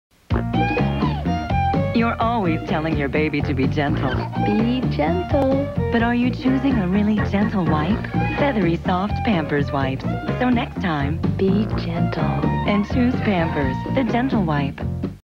Pampers Wipes Commercial - 2000
A staple of Daytime TV - recorded off-air in 2000